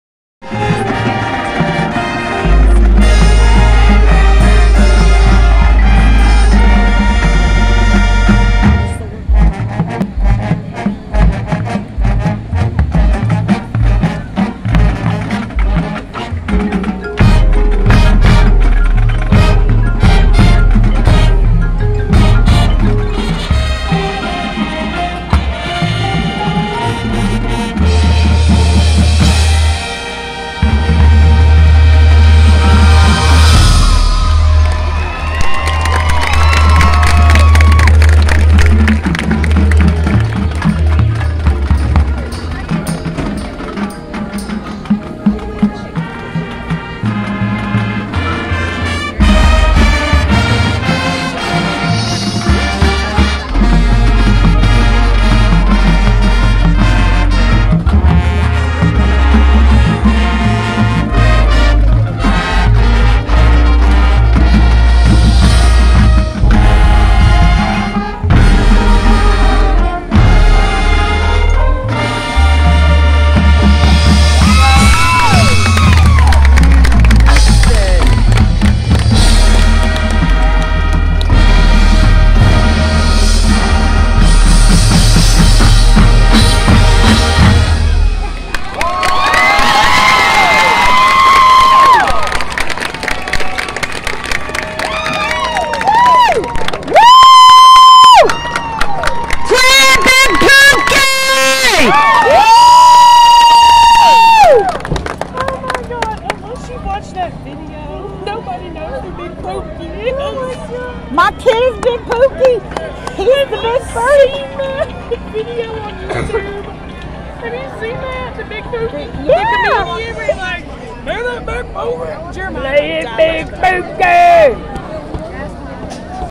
CHEROKEE BAND OF WARRIORS (CBOW) 8/18/21
CBOW Corky Kell Games @
West Forsyth High School
8/18/21
half time part 1.